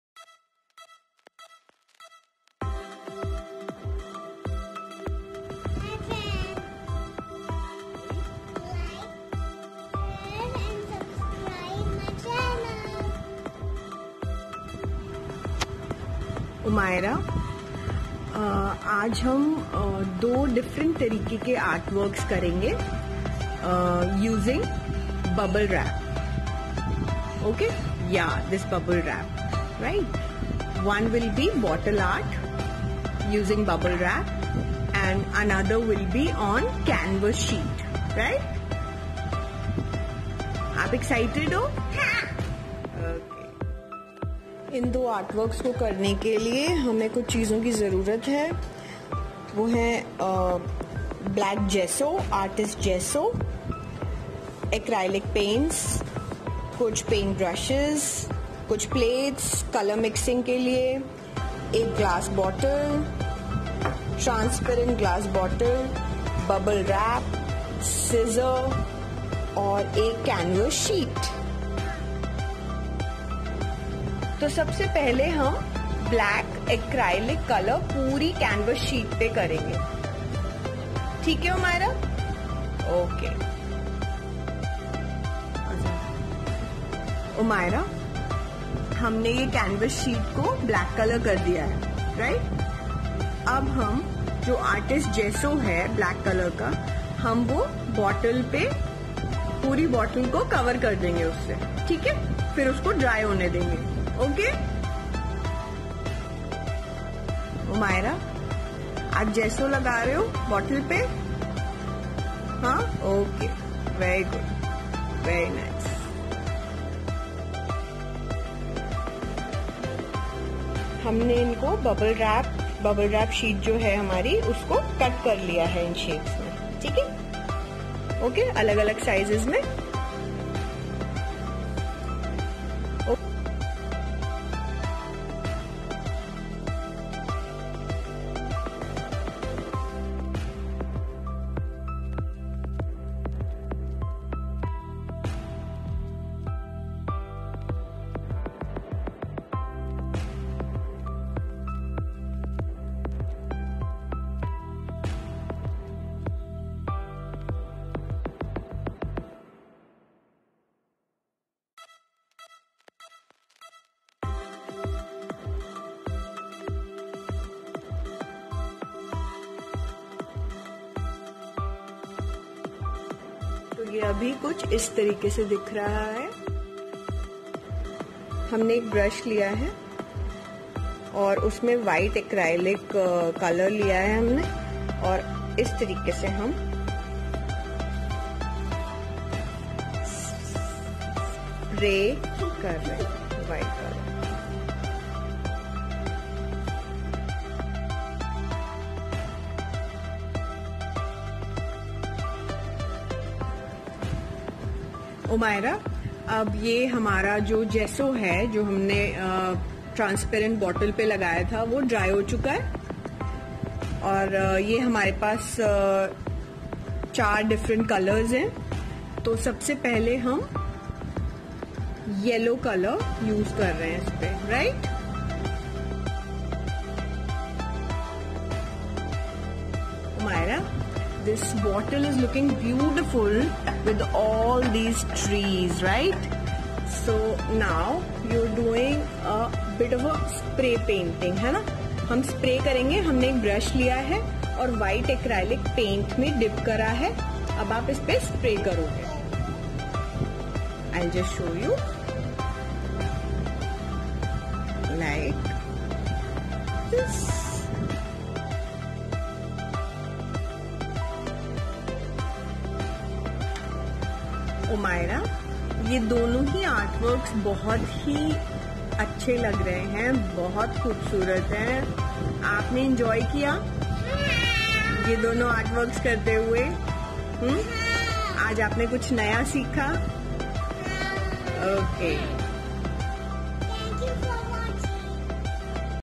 ARTS USING BUBBLE WRAP
Things Required :
 1. Canvas Sheet
 2. Transparent Glass bottle
 3. Acrylic Paints
 4. Black Gesso (Artist Gesso)
 5. Paint Brushes
 6. Bubble wrap sheet (any waste sheet from packaging)
 7. Scissor
 8. Toothbrush for spraying paint
 9. Colour mixing pallete,  plates
 Please watch video for the methods..